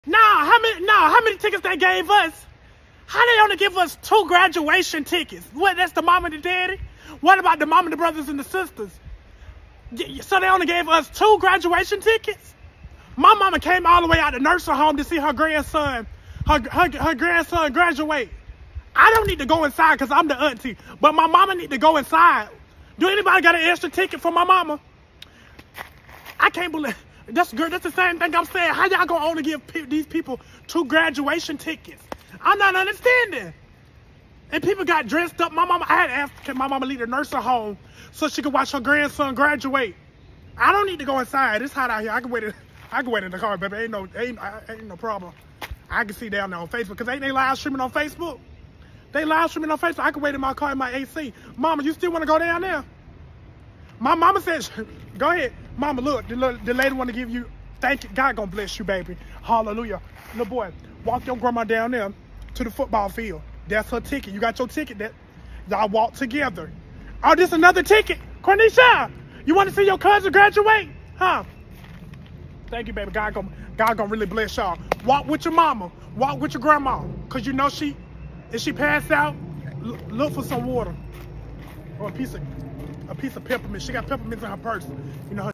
Pov: How the hood Auntie act when the School gives out only 2 Graduation tickets.🎓
How the hood Auntie act when the School gives out only 2 Graduation tickets.